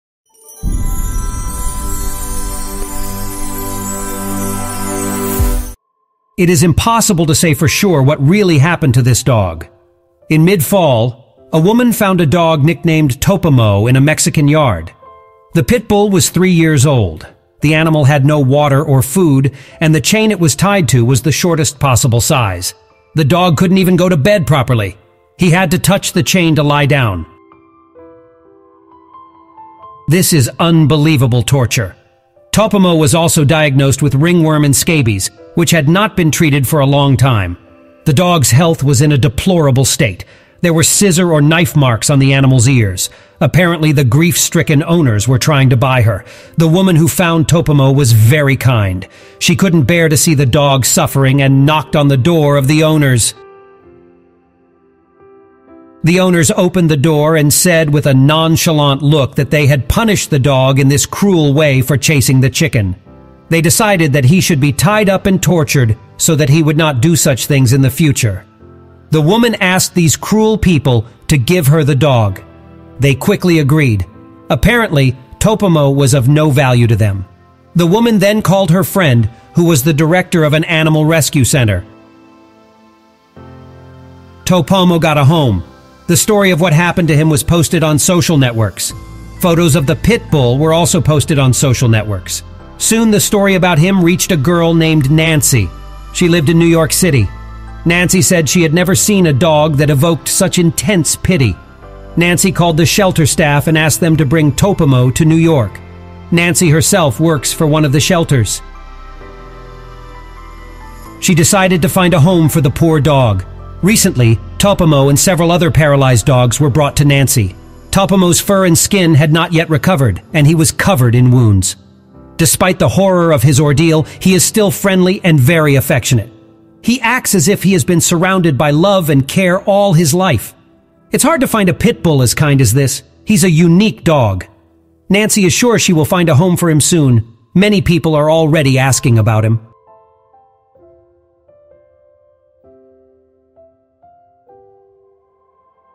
The Starving Dog Leaned His Muzzle Against the Short Chain to Sleep
It is impossible to say for sure what really happened to this dog. In mid-fall, a woman found a dog nicknamed Topomo in a Mexican yard.